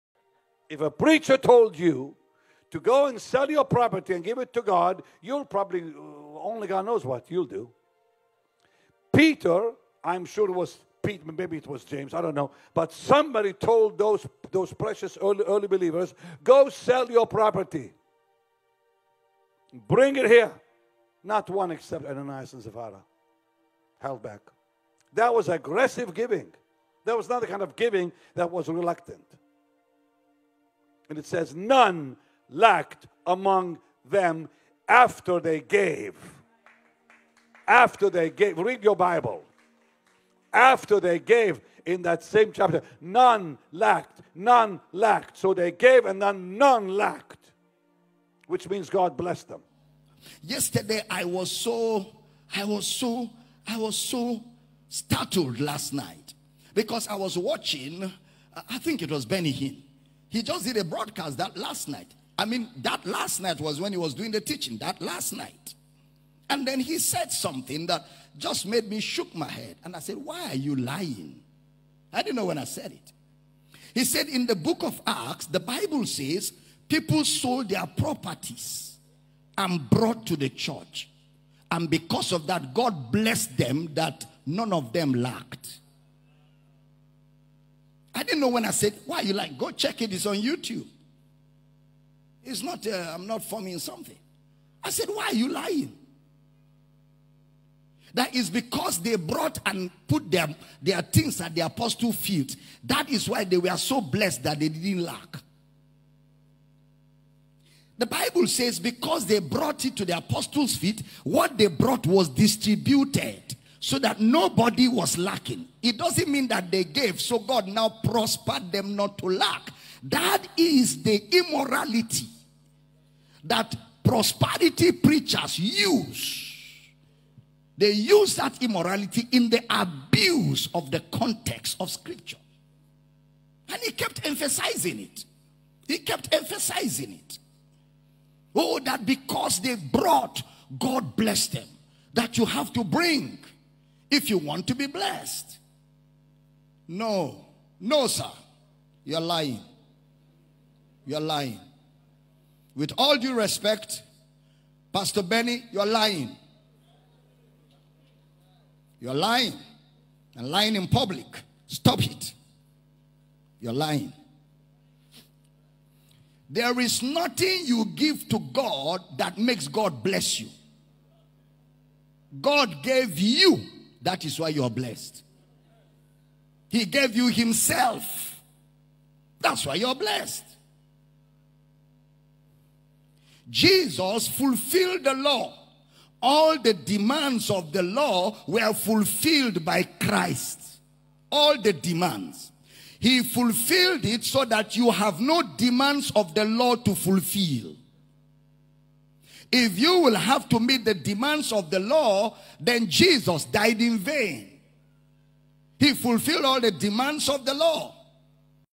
PASTOR BENNY HINN YOU ARE LYING - Dr. Abel Damina
Dr.Abel Damina accuse Benny Hinn of twisting the scriptures for monetary gain.